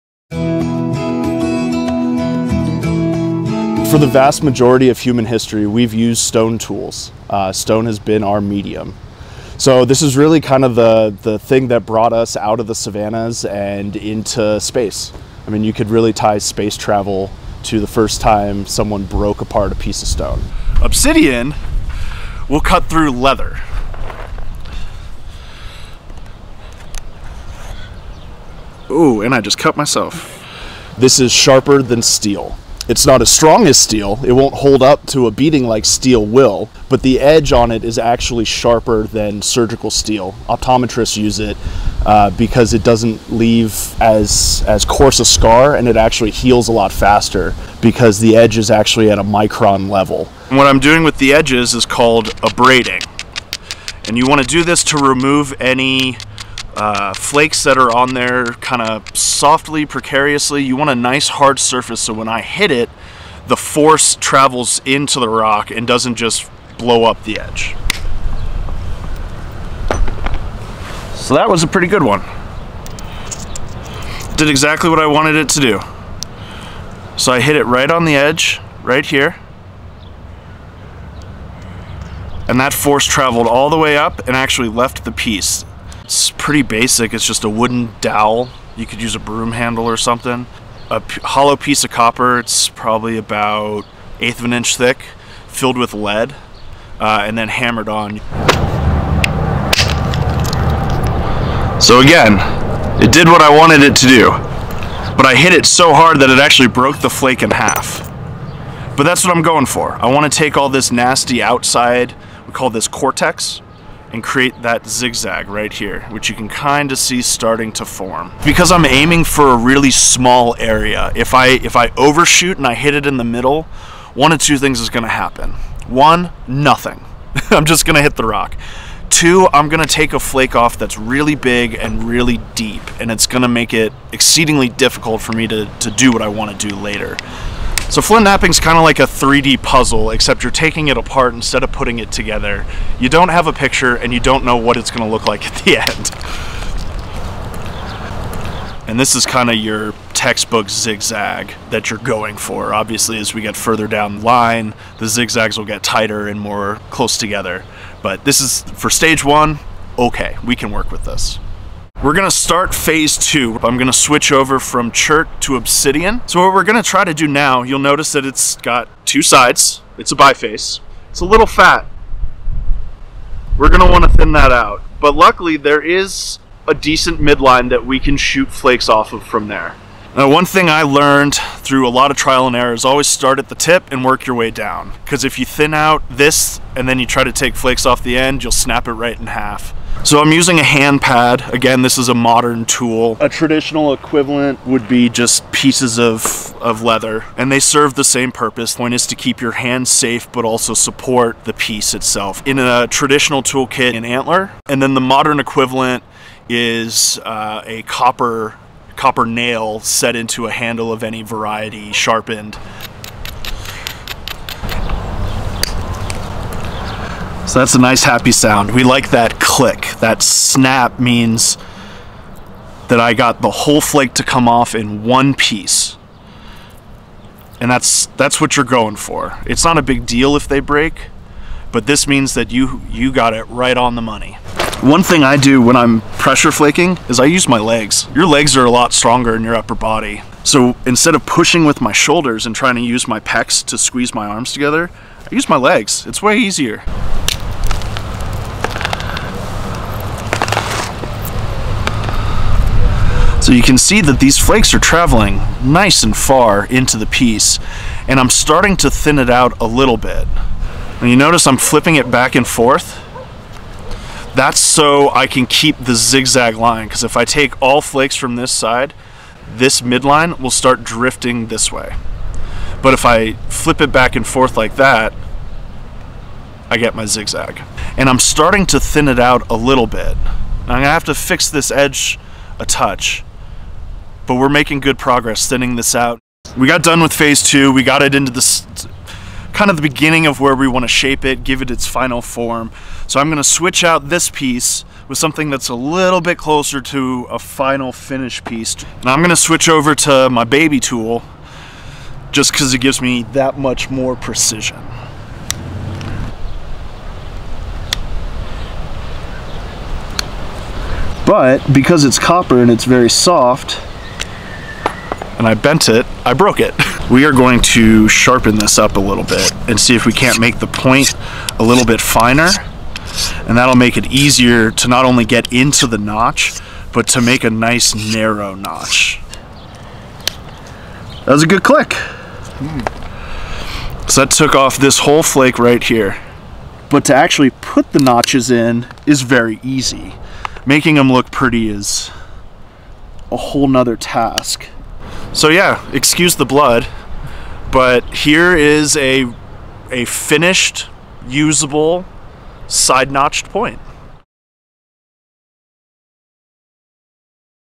Flint Knapping
Humans used stone tools for 99% of their existence, and the process of making stone tools is called flint knapping. In this video, Alex Craib demonstrates the process of flint knapping from unaltered stone to completed stone tool.